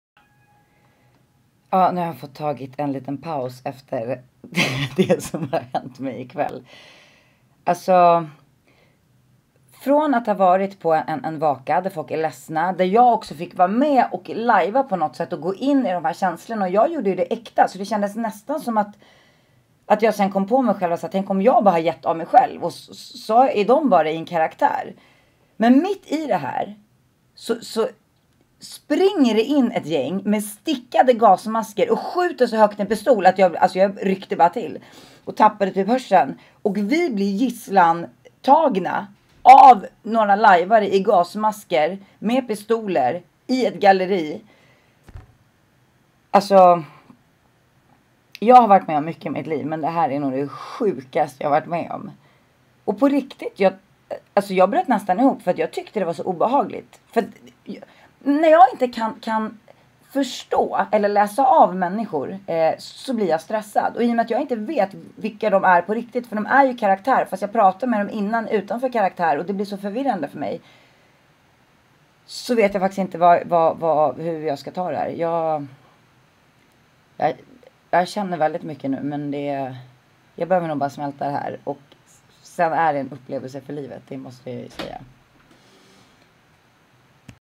Sanna Bråding om lajvet i Köpenhamn
Sanna landar och reflekterar över det som hände på lajvet i Köpenhamn.